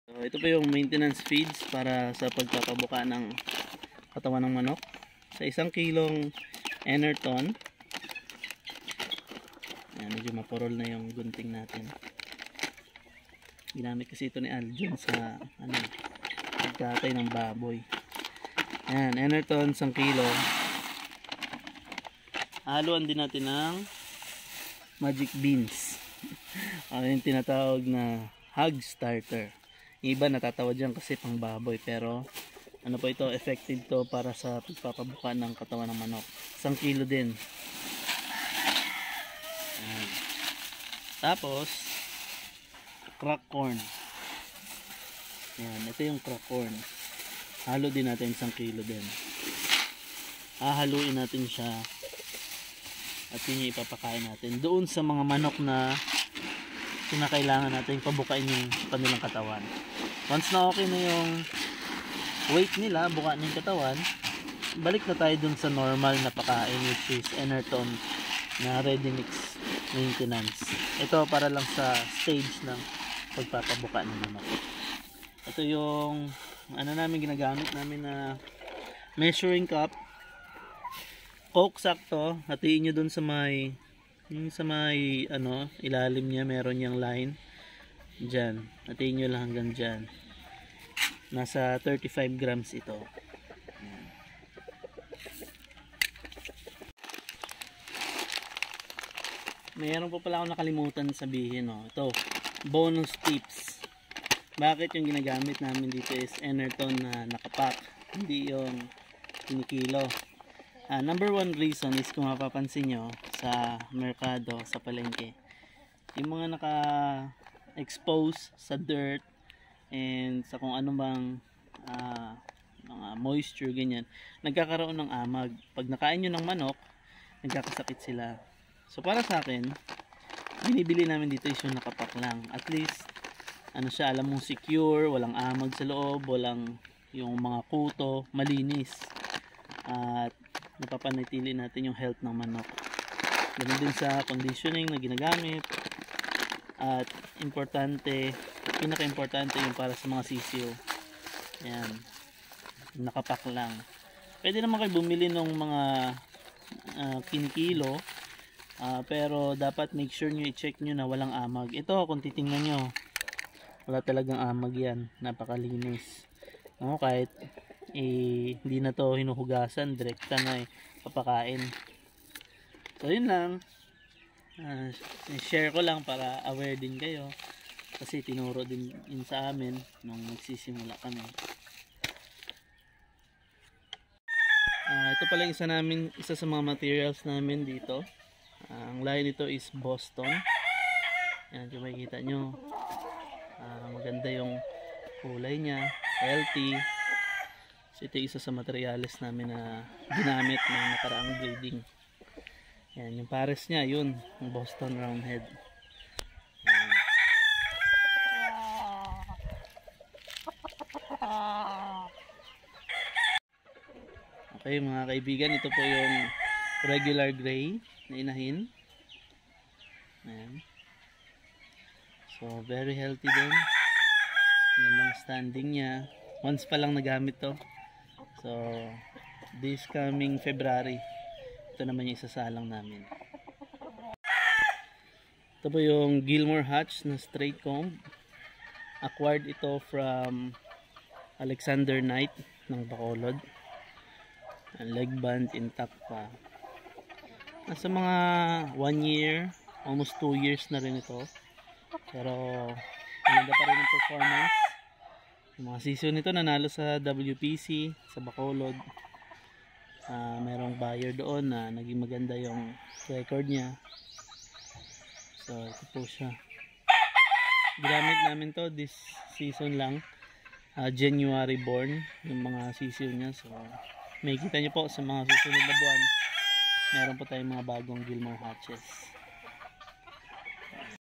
Tamang timpla ng maintenance feeds sa backyard manukan
Tamang timpla ng maintenance feeds para sa backyard manukan.

introduction of pullet materials

boston
regular grey
5k sweater
cardinal